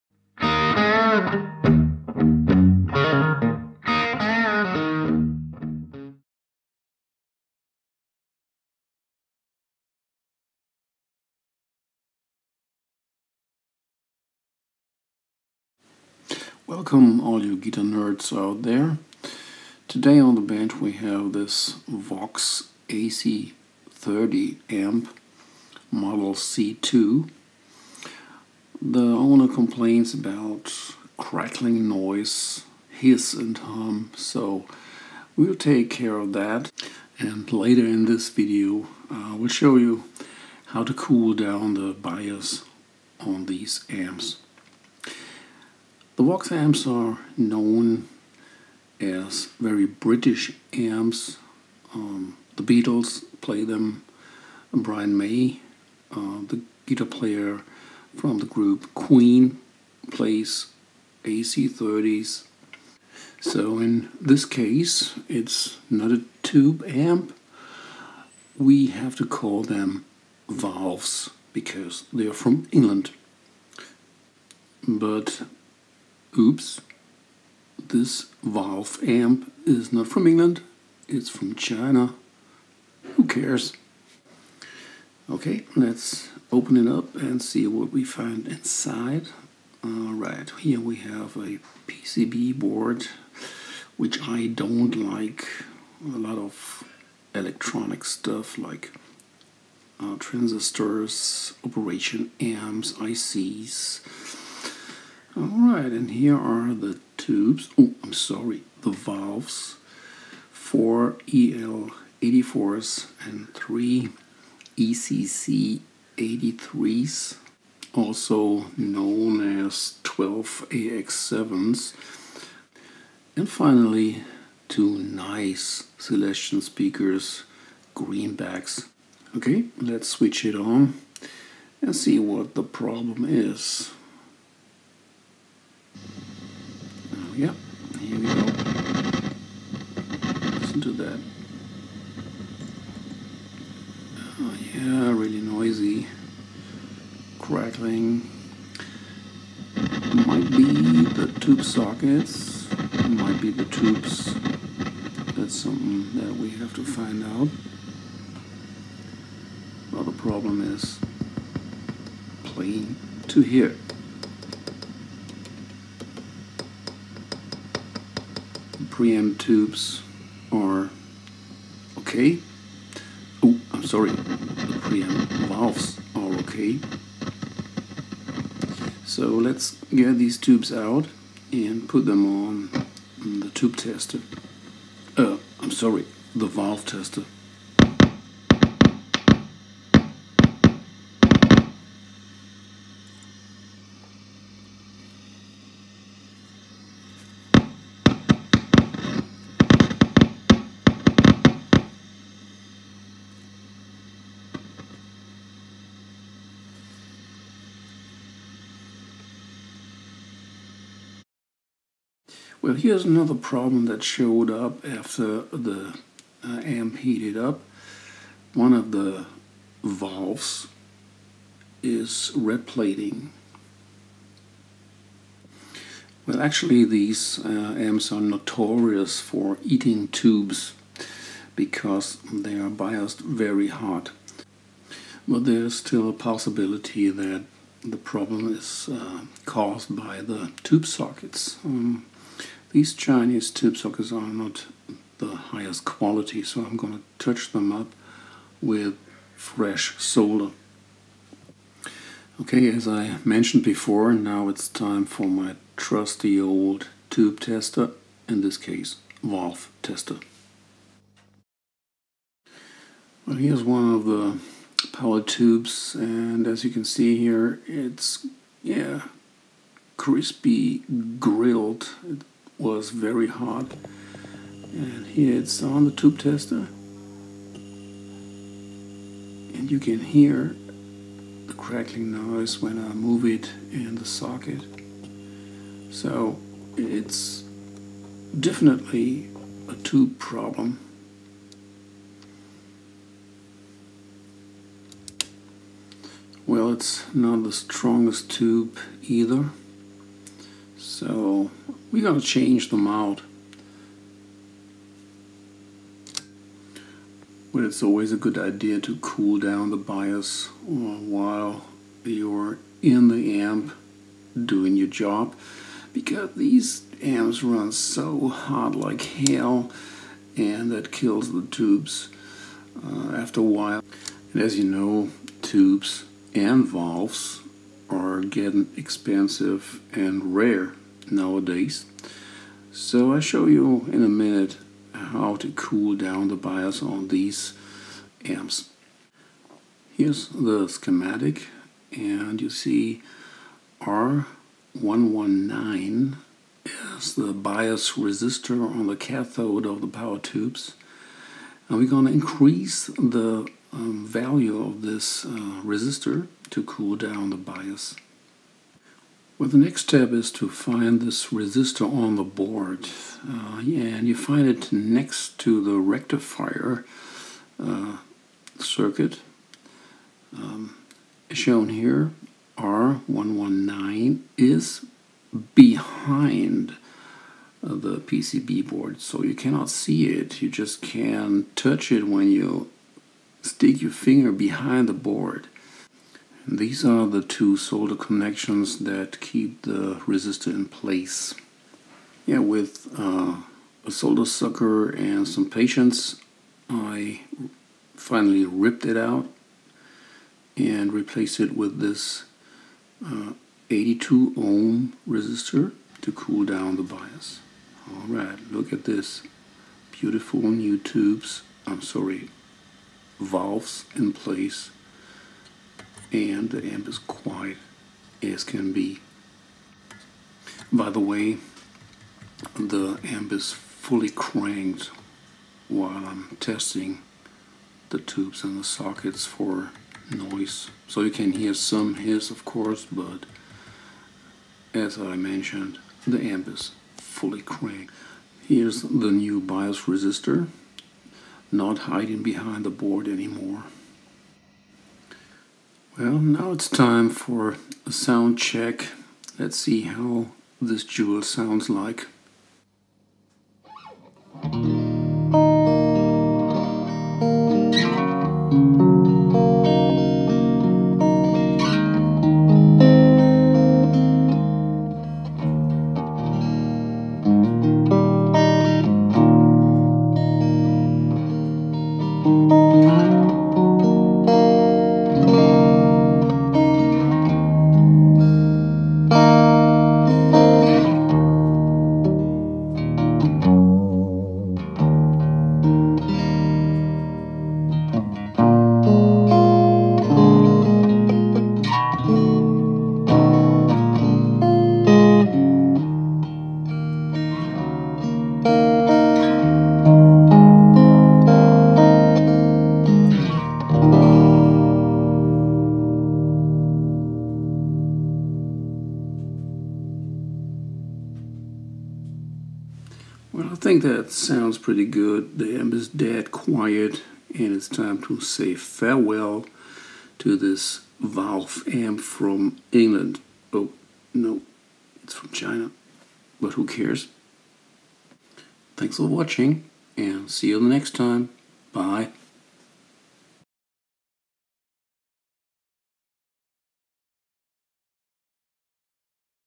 Vox AC 30 C2 how to fix crackling noise and cool down the bias
In times , where tubes get more and more expensive and sometimes even rare, it’s a smart move, to cool down the bias. Vox AC 30 amps run way too hot and that kills the output tubes. This video shows, how to increase the value of the cathode resistor to cool the plate dissipation. Extend tubelife, without changing the tonal characteristics of this legendary amp.